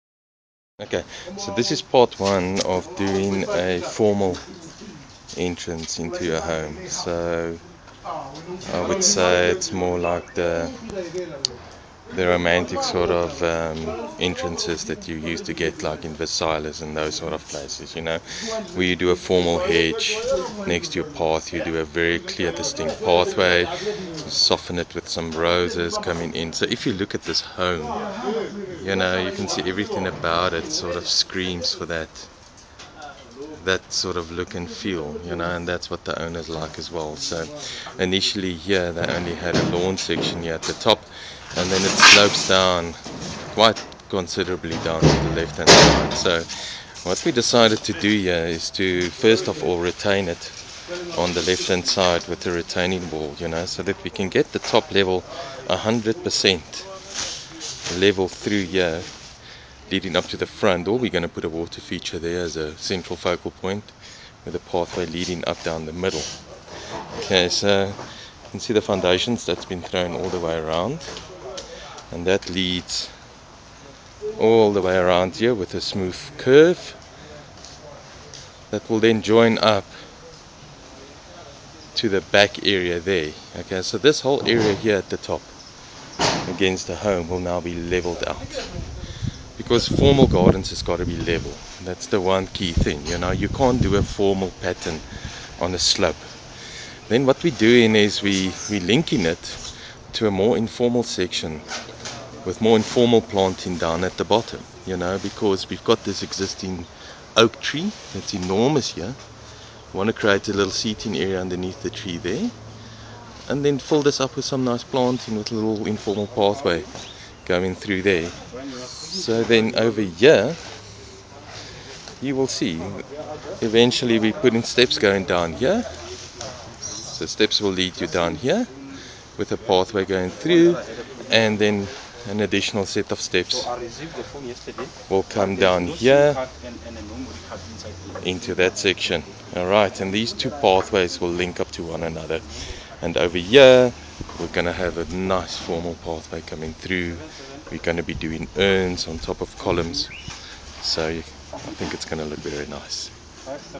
Formal Garden Entrance Part 1
We Build a retainer to get the area at the front door of this home perfectly level. We are going formal at the top and informal at the bottom.